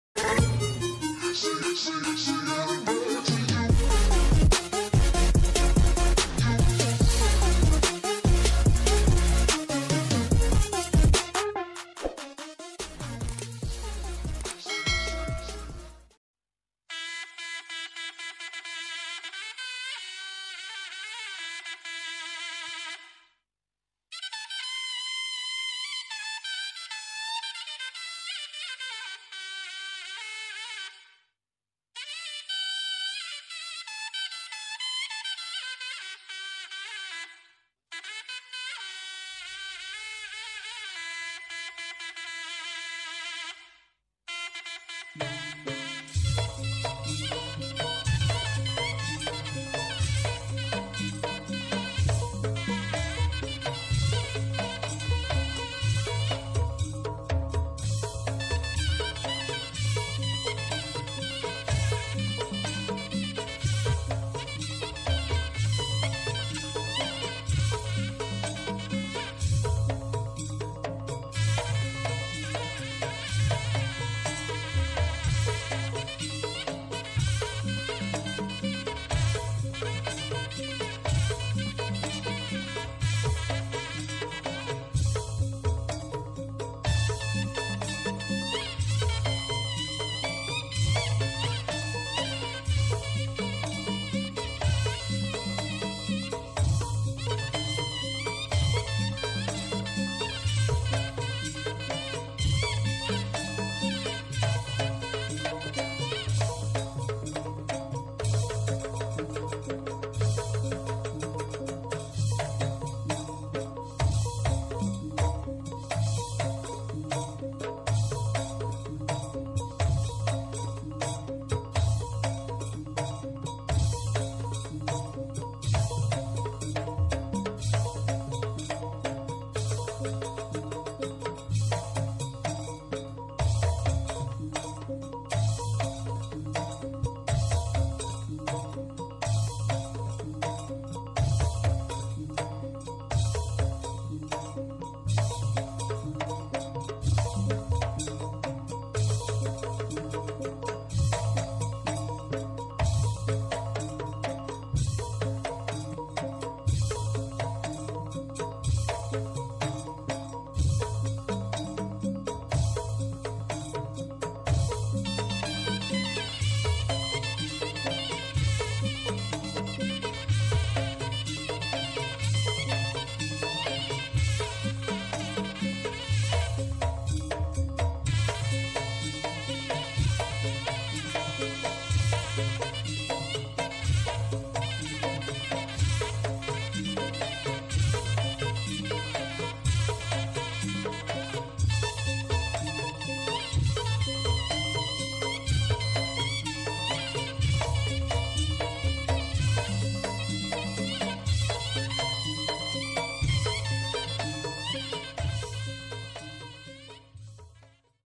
Kerapan Sapi//Spesial Top Speed Tim Banyu Biru
Top speed dari tim banyu biru Bangkalan yang mantap dan jos